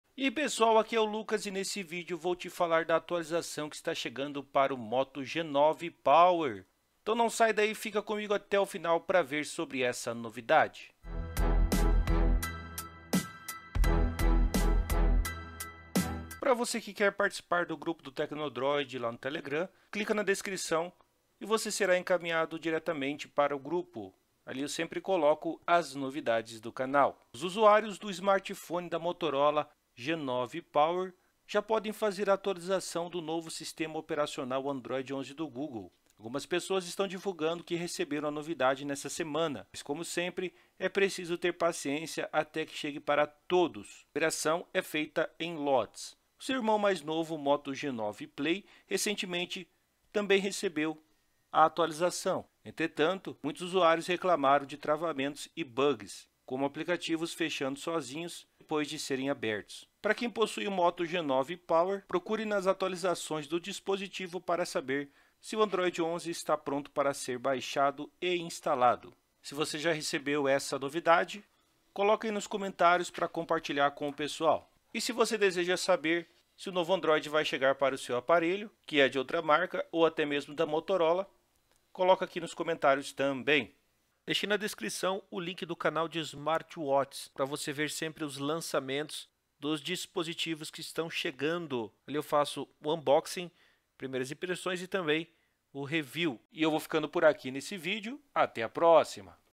Moto G9 Power: Android 11 agora está disponível para instalar
Veja nesse vídeo que o novo sistema operacional Android 11 agora está disponível para o Moto G9 Power, veja as novidades!

PROMOÇÃO AMAZON MOTO G9 POWER